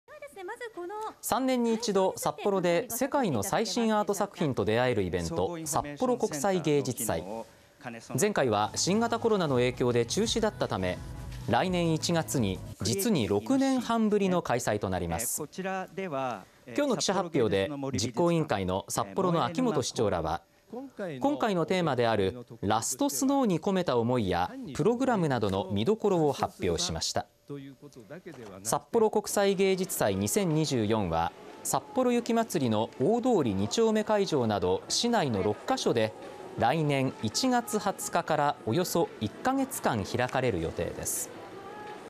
６年ぶり開催 世界の最新アートに出会える「札幌国際芸術祭」来年１月２０日～さっぽろ雪まつり会場など
来年１月に初めて冬に開催される札幌国際芸術祭。参加アーティストやプログラムなど６日に詳細が発表されました。

３年に一度、札幌で世界の最新アート作品と出会えるイベント「札幌国際芸術祭」。前回は新型コロナの影響で中止だったため、来年１月に実に６年半ぶりの開催となります。
６日の記者発表で実行委員会の秋元札幌市長らは今回のテーマである「ＬＡＳＴ ＳＮＯＷ」に込めた思いやプログラムなどの見どころを発表しました。
「札幌国際芸術祭２０２４」はさっぽろ雪まつりの大通２丁目会場など市内の６カ所で、来年１月２０日からおよそ１カ月間開かれる予定です。